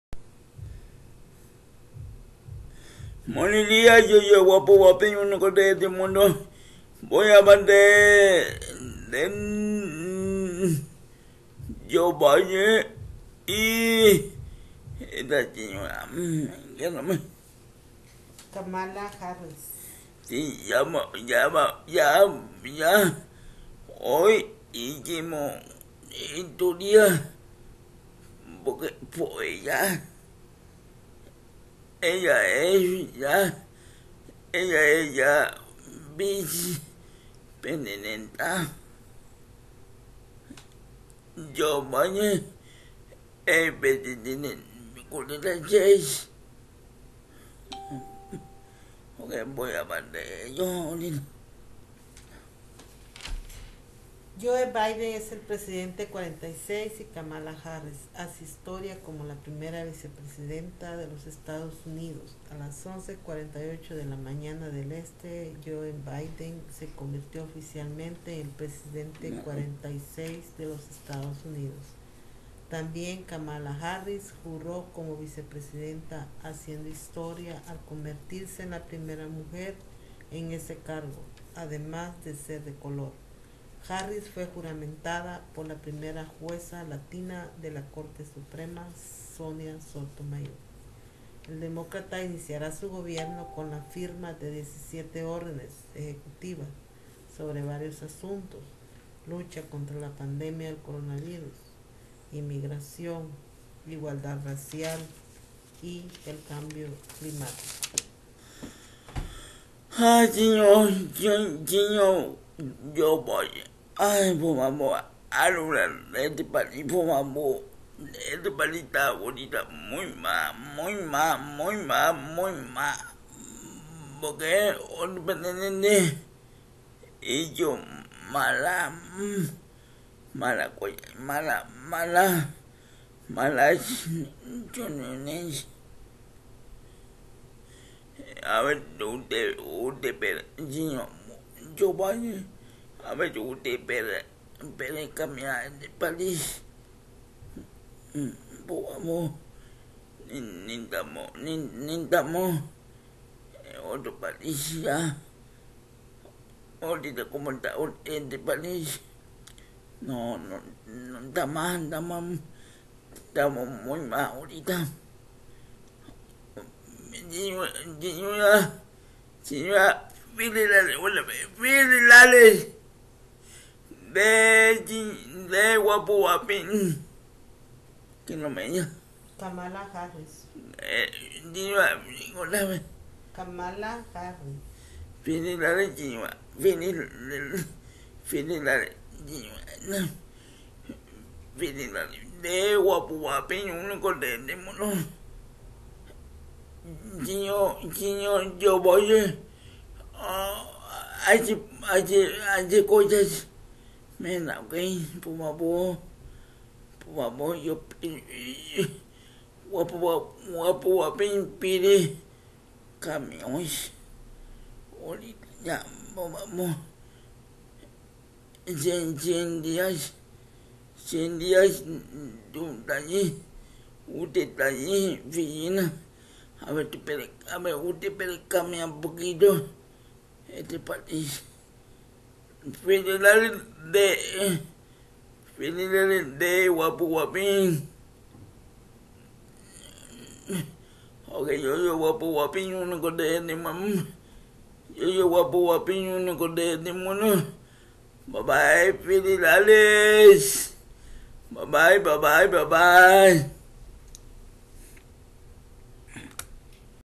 joebiden es el presidente 46 y kamalaharris como vicepresidenta
yo ablo hasi porque naci con paralisis cerebral